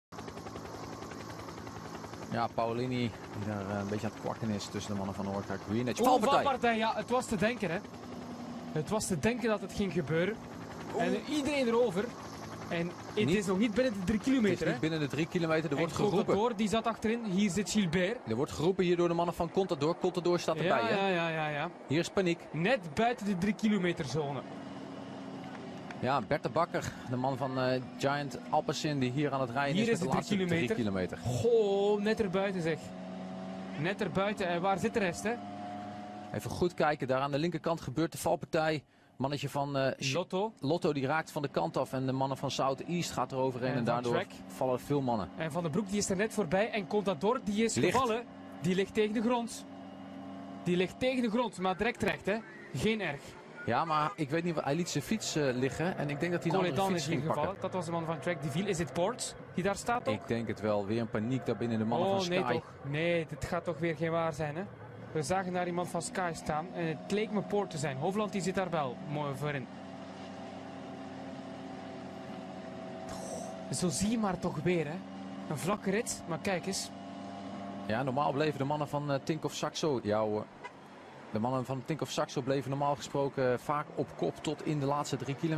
Crash in the peloton - 3,2 km to go - Stage 13 - Giro d'Italia 2015
Crash 3,2 KM - Stage 13 - Giro d'Italia 2015